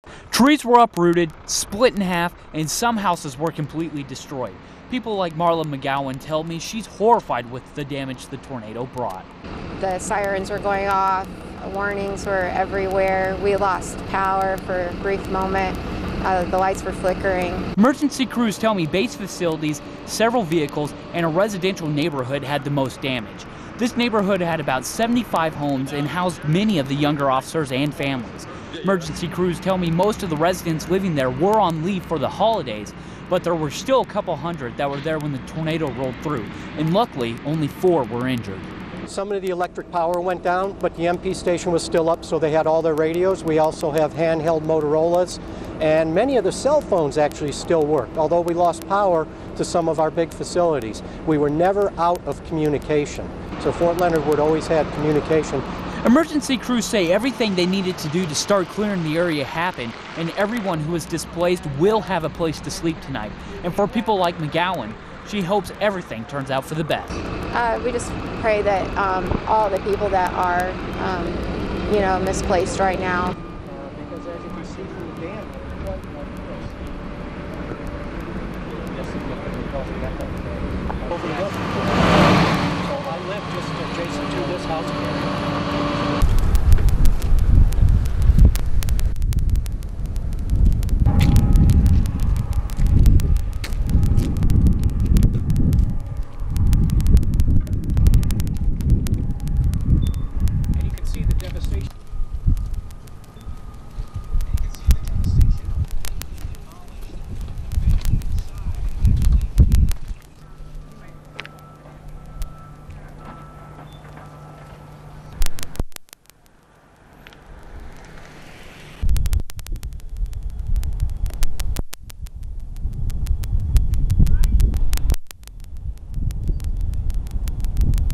Fort Leonard Wood Tornado
I couldn't believe the extent of the damage after traveling Fort Leonard Wood in the Ozarks. This was my first real sight of the damage that mother nature could do.